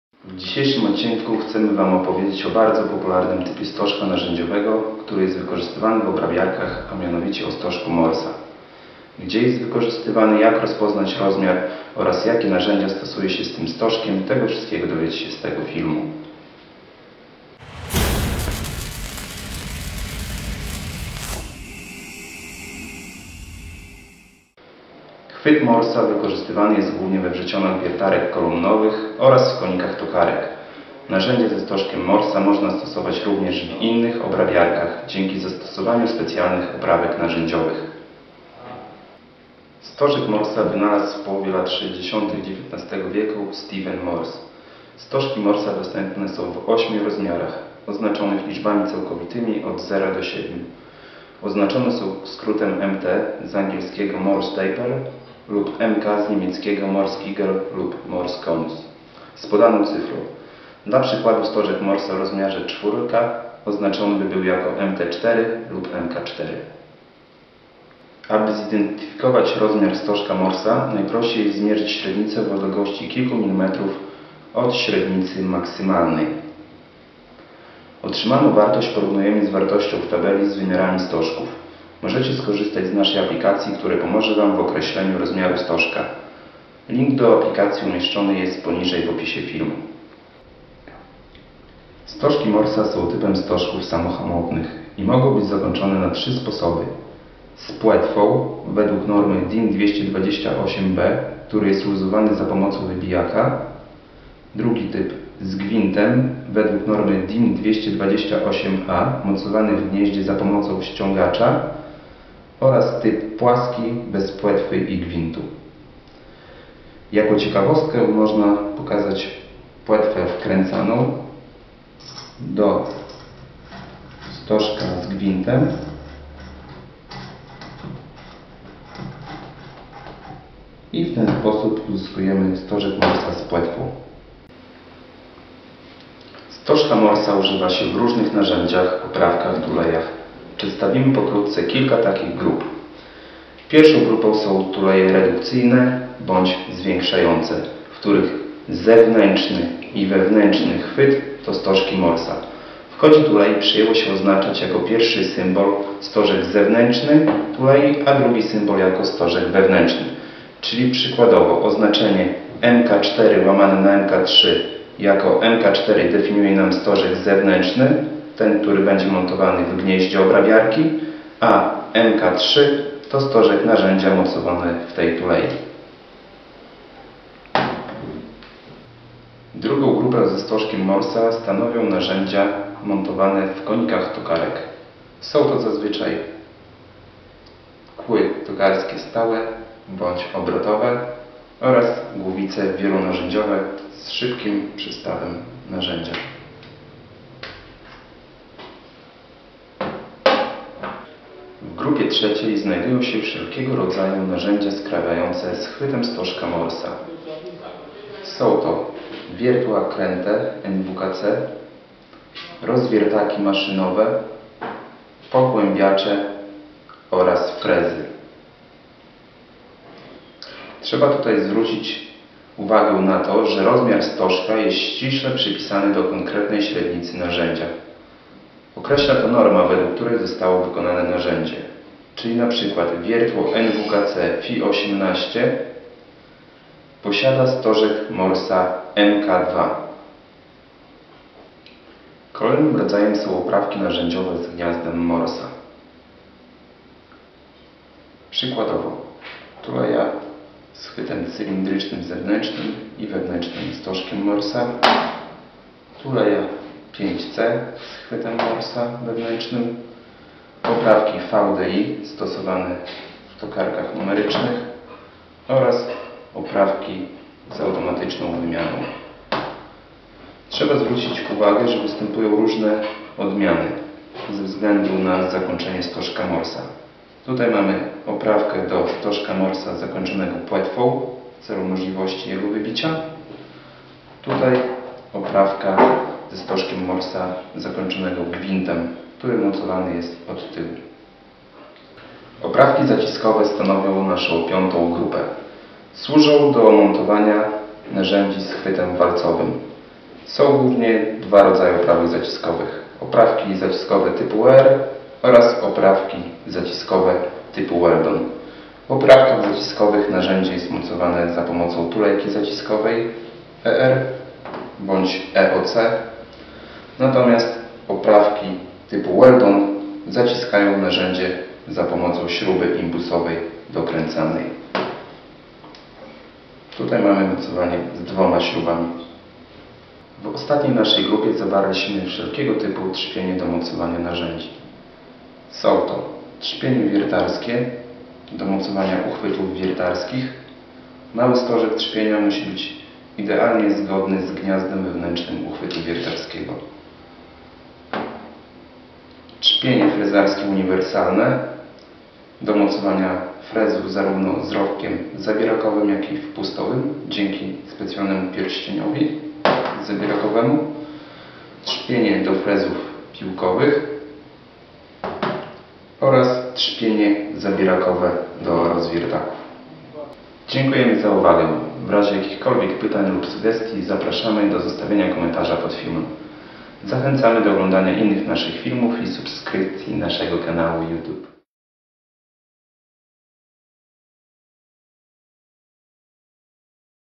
Stożek maszynowy Morse DIN 228
Film przedstawiający opis popularnego stożka maszynowego typu Morse. Chwyt Morse'a jest jednym z najczęściej używanych typów w chwytach wierteł krętych z chwytem stożkowym oraz rozwiertaków maszynowych, we wrzecionach wiertarek pionowych oraz konikach tokarek. Poniżej link do programu, który pomoże wam określić rozmiar stożka Morse'a.